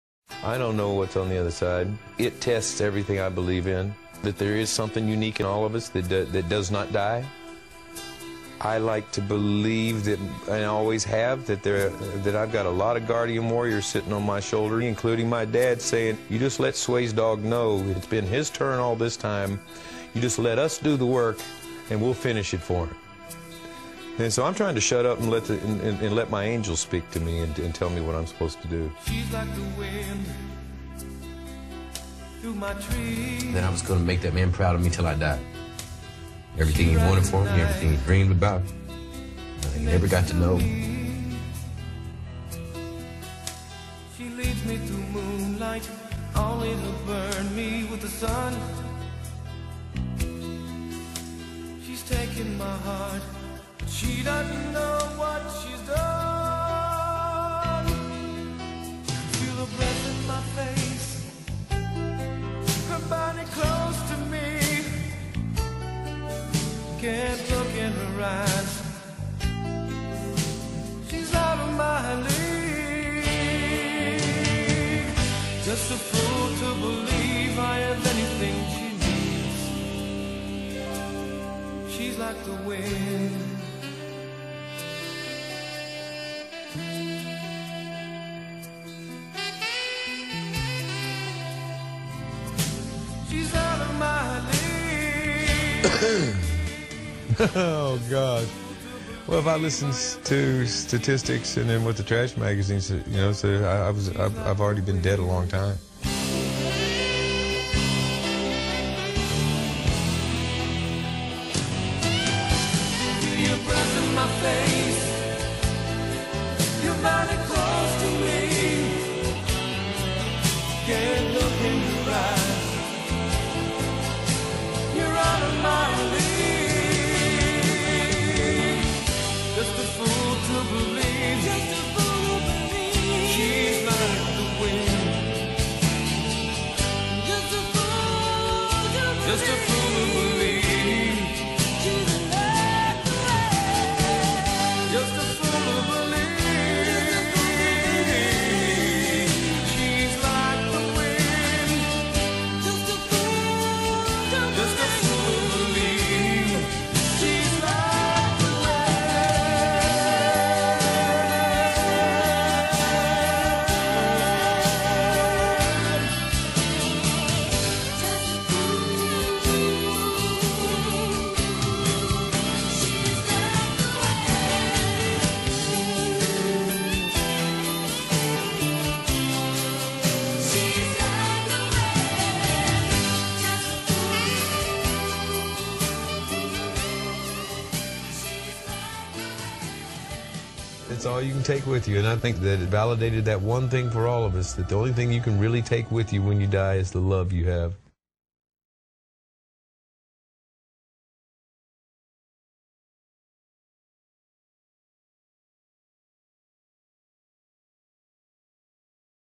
98.1 CHFI remembers Patrick Swayze
Patrick Swayze has passed away at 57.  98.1 CHFI remembers him with this tribute video.